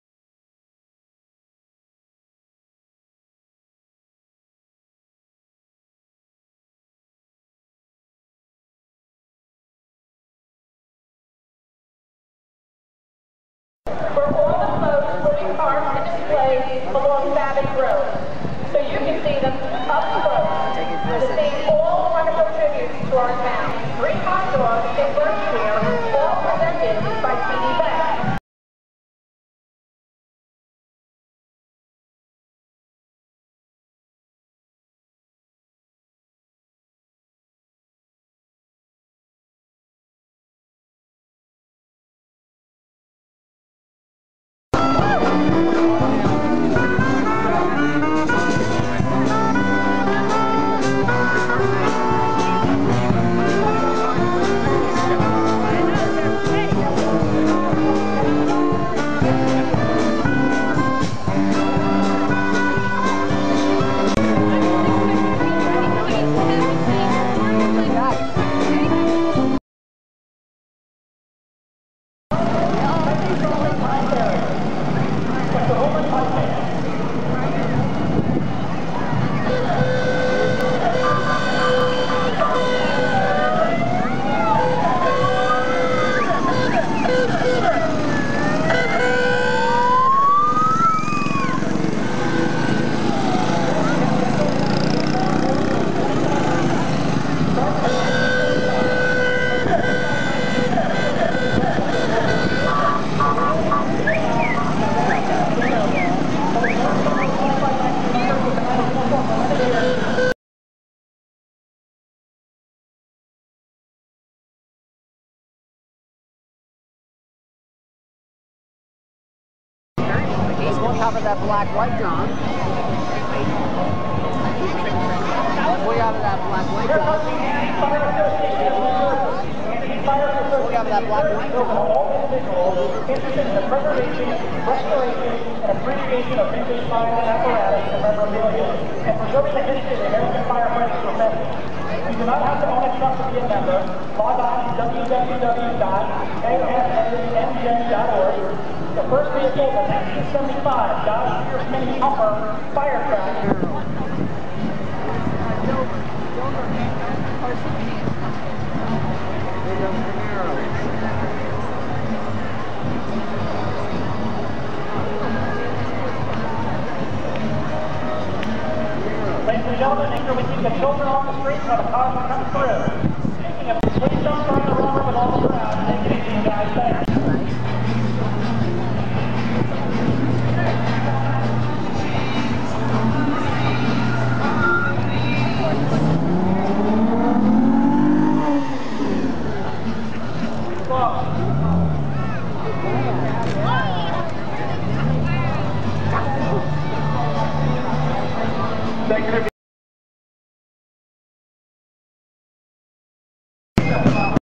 Denville,NJ 100th Anniversary Parade
Pictures and Video from Denville,NJ 100th Anniversary Parade held in June,2013